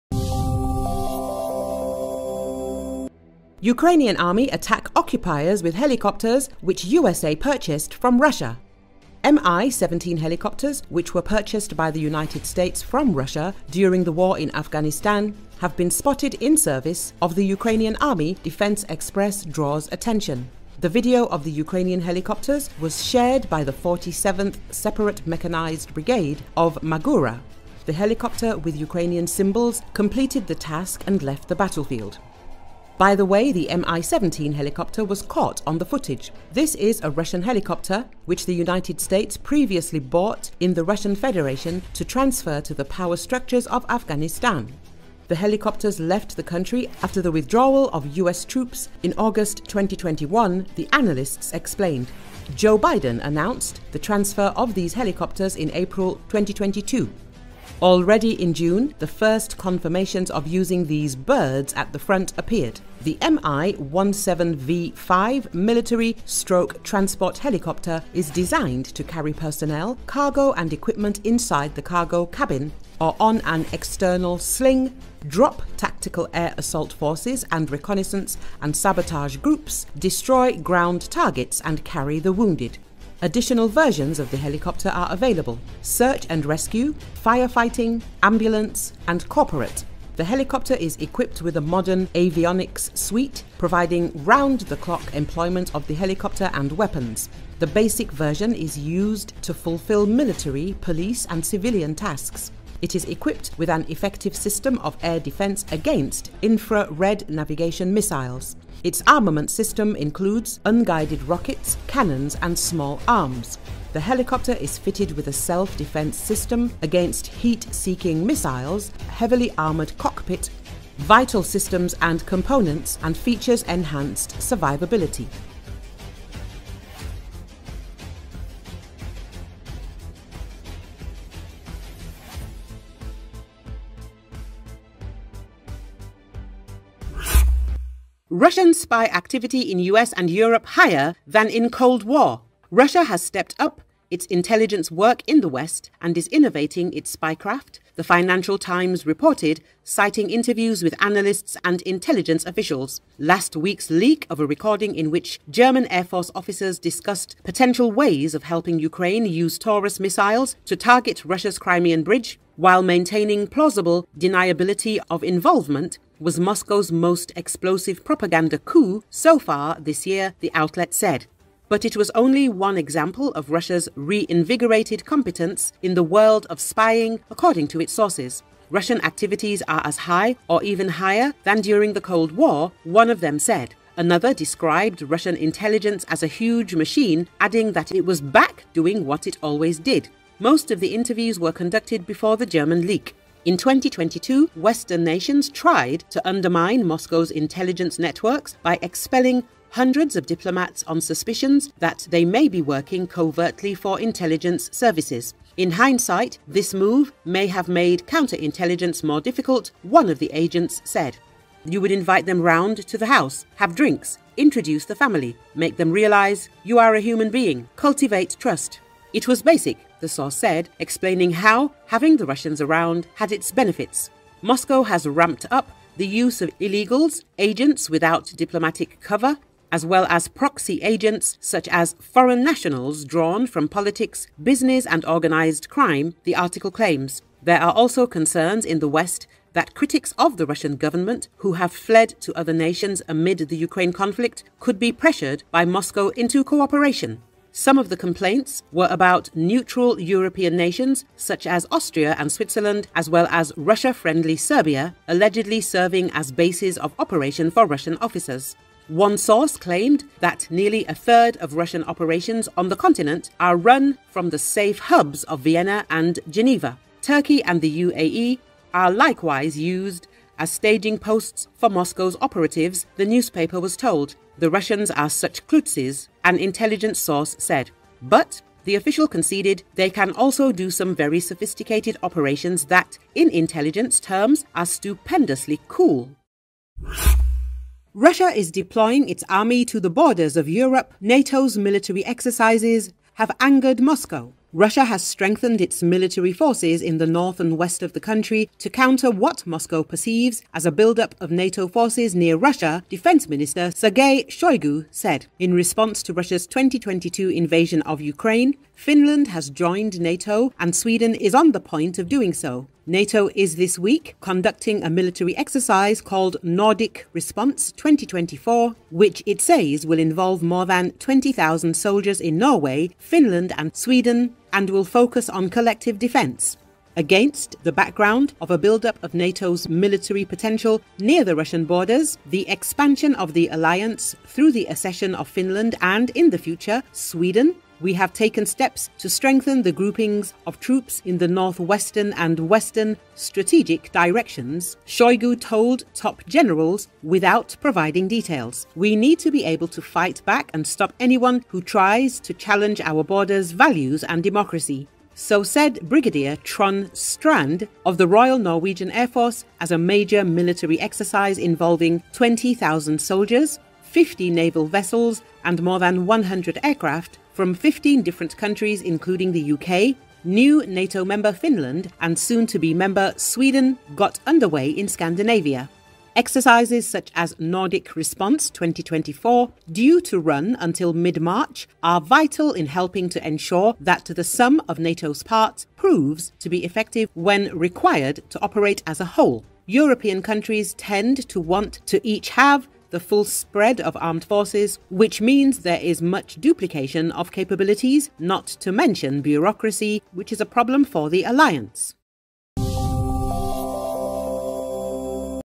Ukrainian Army attack occupiers with helicopters, which USA purchased from Russia
Mi-17 helicopters, which were purchased by the United States from Russia during the war in Afghanistan, have been spotted in service of the Ukrainian Army, Defense Express draws attention.
The video of the Ukrainian helicopters was shared by the 47th separate mechanised brigade of "Magura". The helicopter with Ukrainian symbols completed the task and left the battlefield.
"By the way, the Mi-17 helicopter was caught on the footage. This is a Russian helicopter, which the United States previously bought in the Russian Federation to transfer to the power structures of Afghanistan. The helicopters left that country after the withdrawal of US troops in August 2021," the analysts explained.
Joe Biden announced the transfer of these helicopters in April 2022. Already in June, the first confirmations of using these "birds" at the front appeared.
The Mi-17V-5 military/transport helicopter is designed to carry personnel, cargo and equipment inside the cargo cabin or on an external sling, drop tactical air assault forces and reconnaissance and sabotage groups, destroy ground targets and carry the wounded.
Additional versions of the helicopter are available: search & rescue, fire-fighting, ambulance, and corporate.
The helicopter is equipped with a modern avionics suite providing round-the-clock employment of the helicopter and weapons. The basic version is used to fulfil military, police and civilian tasks. It is equipped with an effective system of air defence against infra-red-navigation missiles. Its armament system includes unguided rockets, cannons and small arms.
The helicopter is fitted with a self-defense system against heat seeker missiles, heavily-armored cockpit, vital systems and components, and features enhanced survivability.